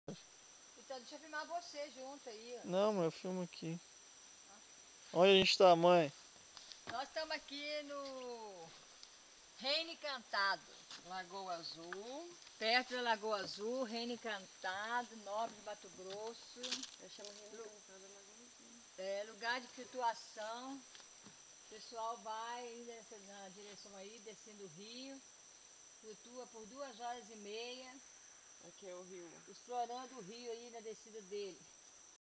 Rio Saloba (Lagoa Azul - Reino Encantado) - Nobres/M
Os peixes do limpido rio Saloba.